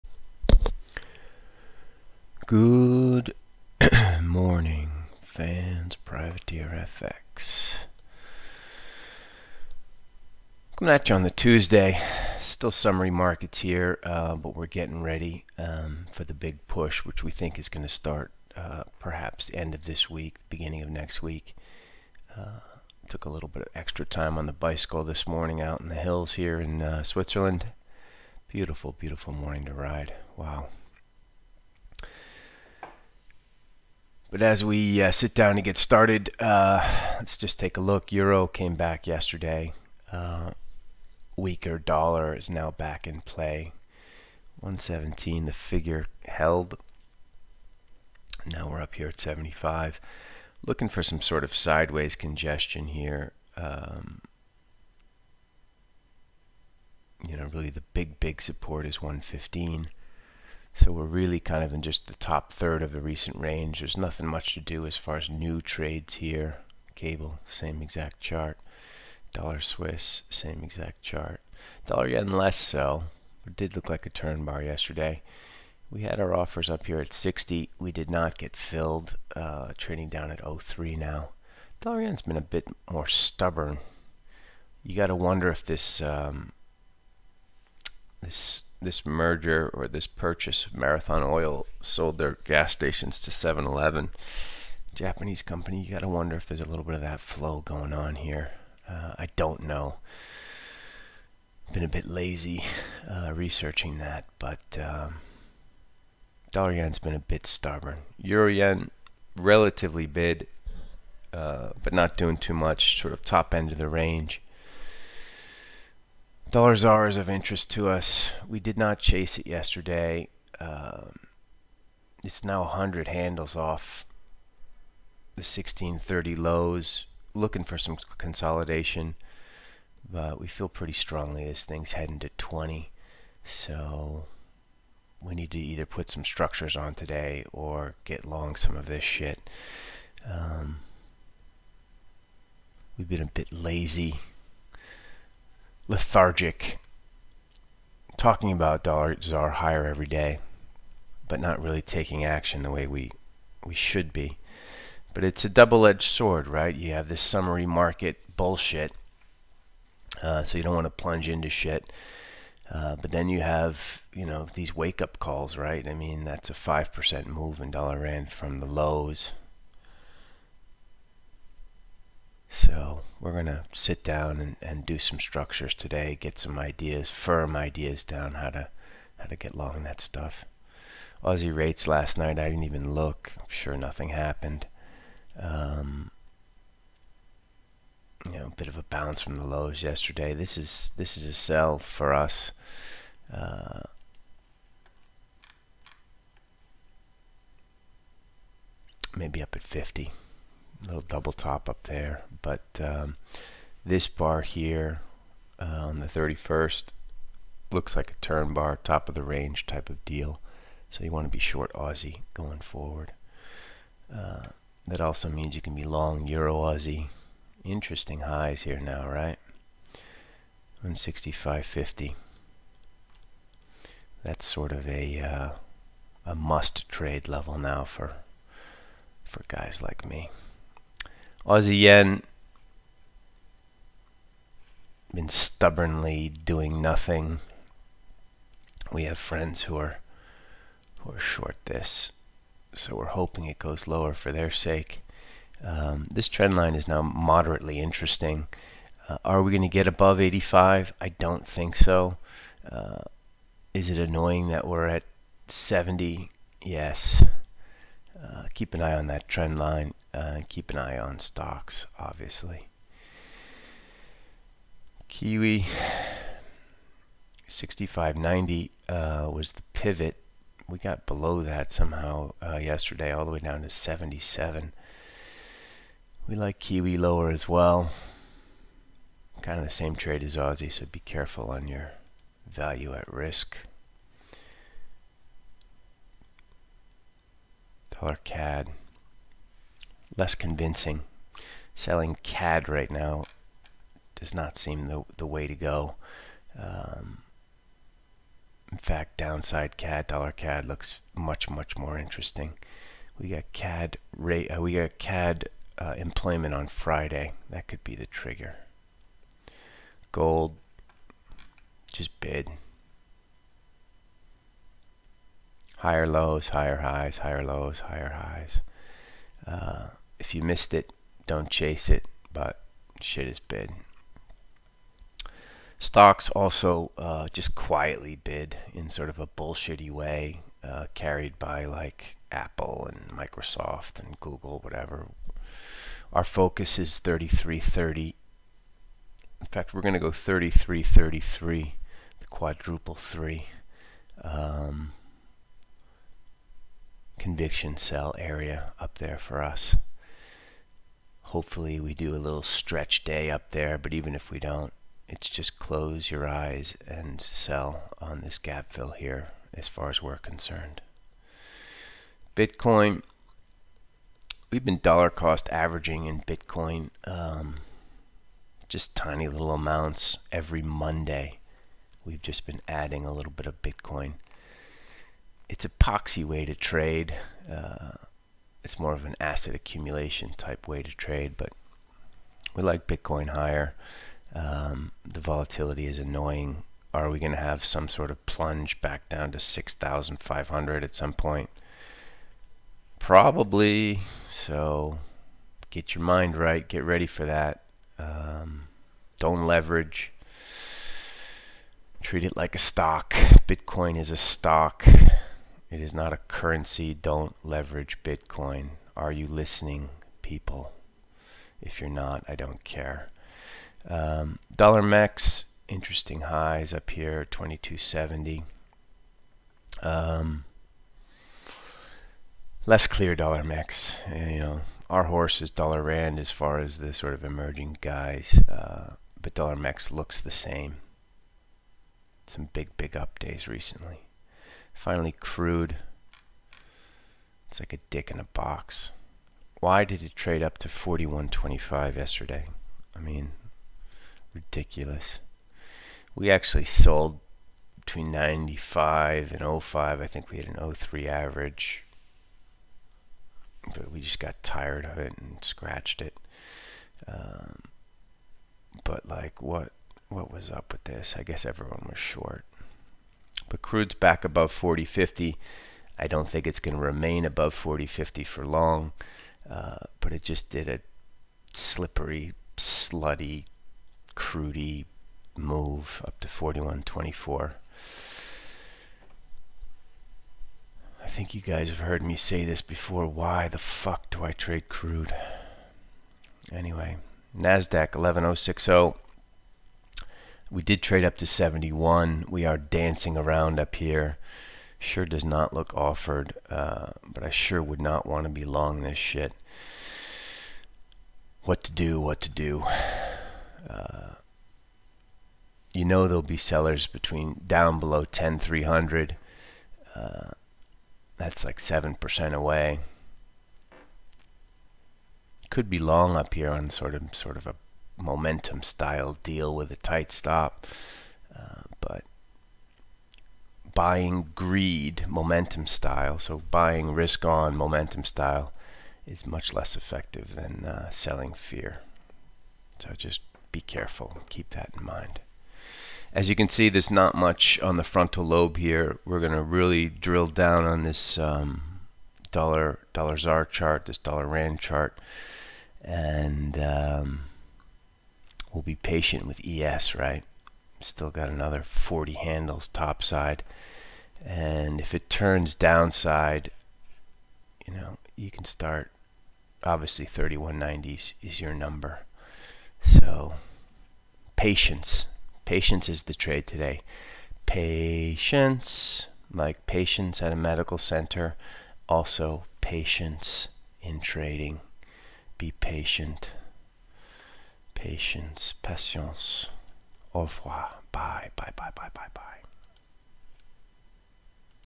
Forex Trader's Morning Call 04Aug20 [AUDUSD, EURUAD, AUDJPY]
still Summer markets
AUDUSD getting short
EURAUD 'must trade' levels
AUDJPY interesting
USDJPY a little stubborn
USDZAR move up?
Gold bid
SPX 3333
BTC/Bitcoin

Web: PrivateerFX.com
You can help keep this channel afloat (e.g. covering website hosting fees & beer) by clicking on this donations